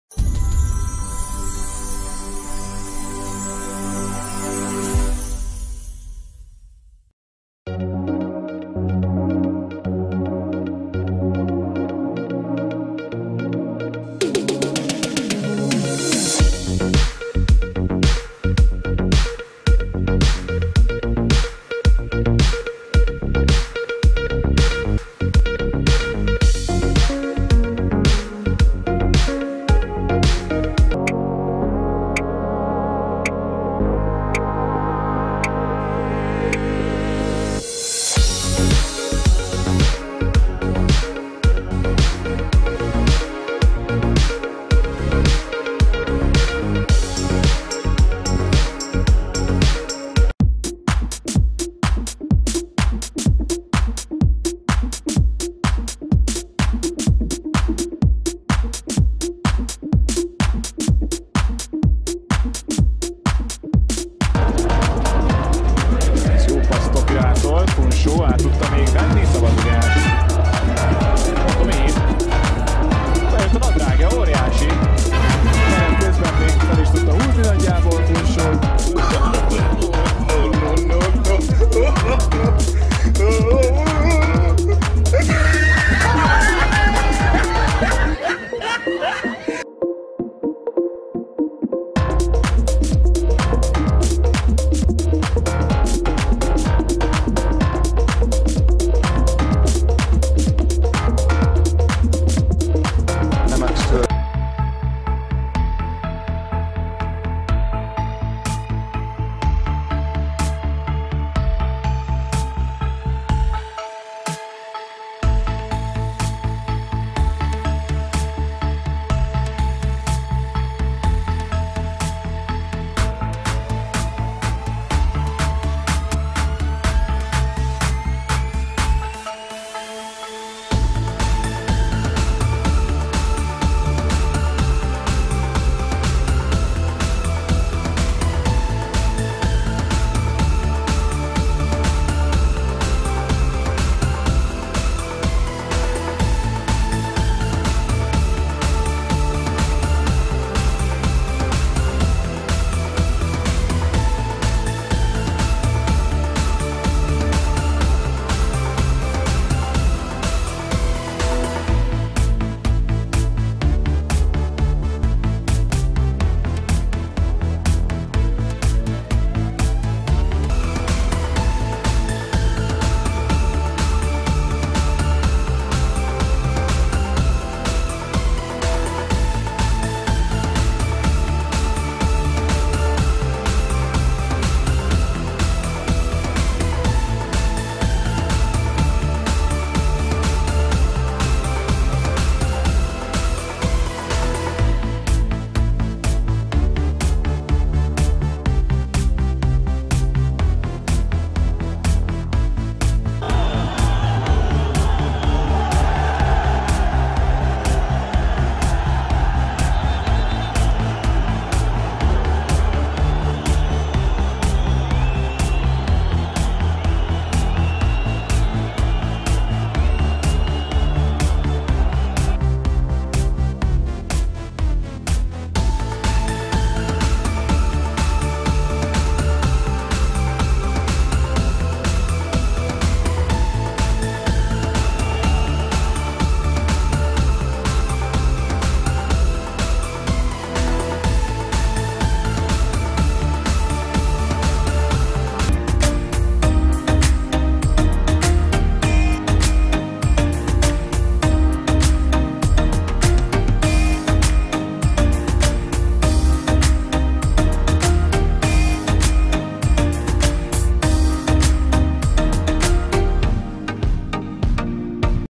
Pantsed Soccers
The moments that soccers get pantsed. Sometimes they joke , sometimes they make it accidentally and sometimes they make it when they angry.  Including vladimirs kamess, mattias swanbrg, jack alnvik and others